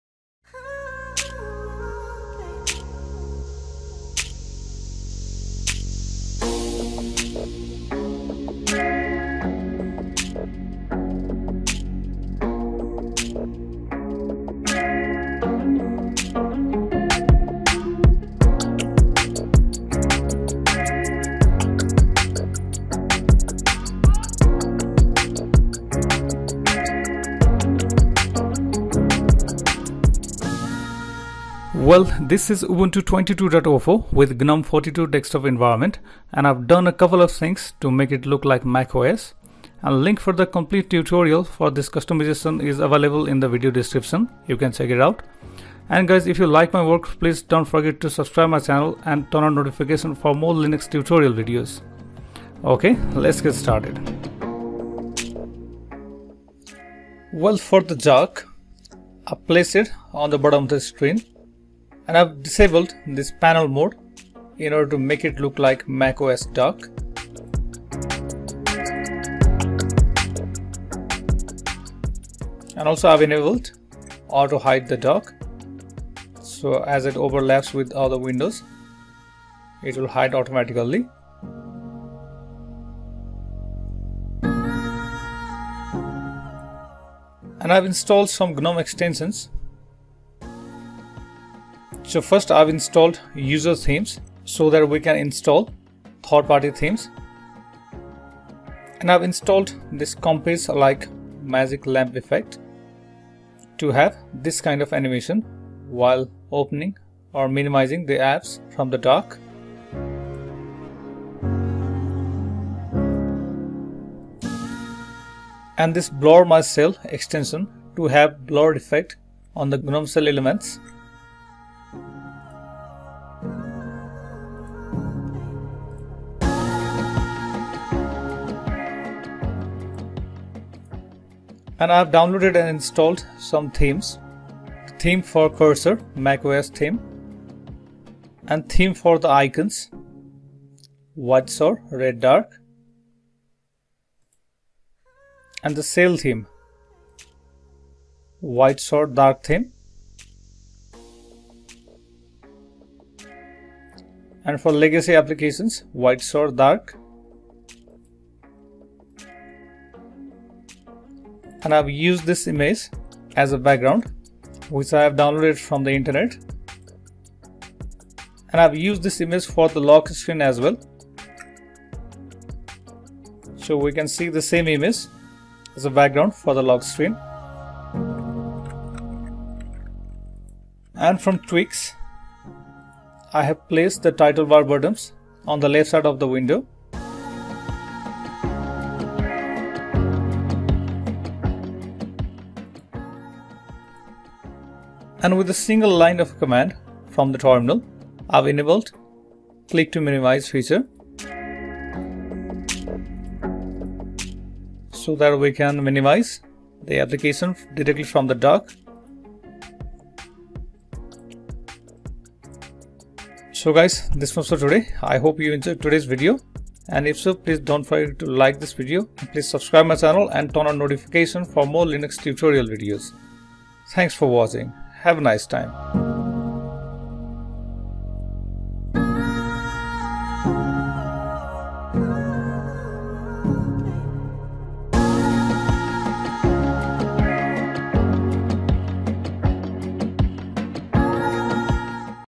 Make Ubuntu Look Like MacOS | Gnome 42 Customization | Ubuntu 22.04 Customization | MacUbuntu
This is ubuntu 22.04 with gnome 42 desktop environment, and I've done a couple of things to make it look like MacOS

Timestamps:
00:00 Final Result
00:30 Intro
00:55 Dock Settings
01:25 Gnome Extensions installed
02:10 Icon, GTK, Cursor Themes
02:38 Wallpaper and Lockscreen background
02:58 Title bar placement
03:13 Click to minimize activation
03:50 Outro